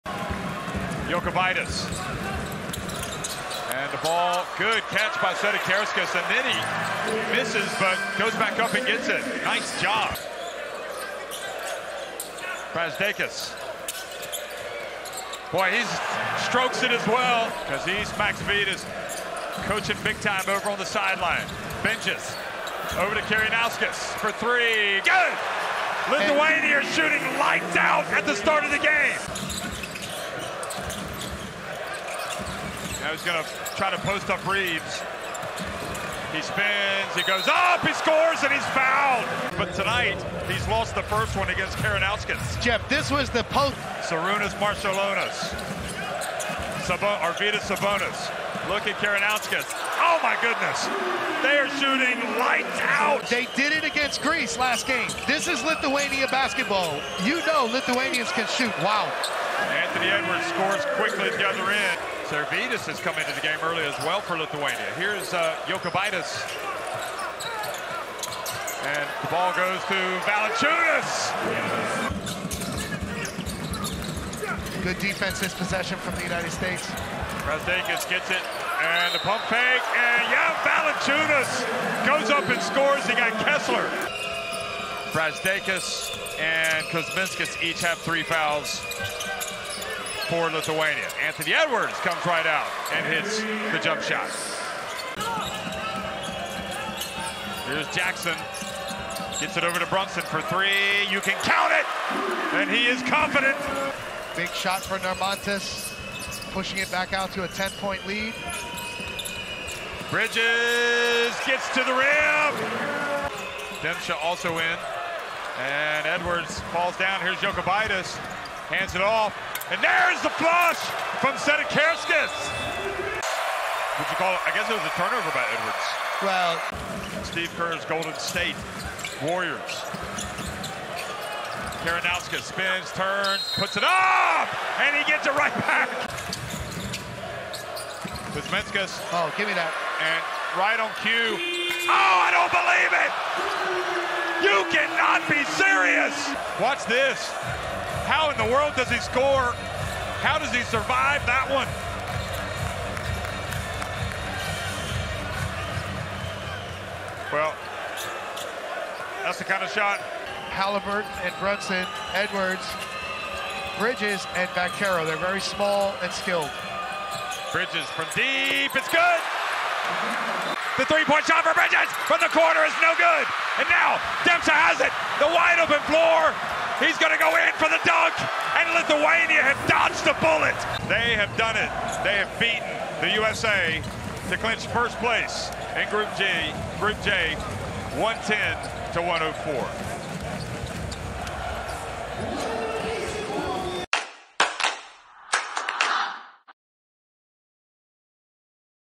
USA 🇺🇸 vs Lithuania 🇱🇹 | J9 Highlights | FIBA Basketball World Cup 2023
Watch the game highlights between United States and Lithuania in the First Round of the FIBA Basketball World Cup 2023.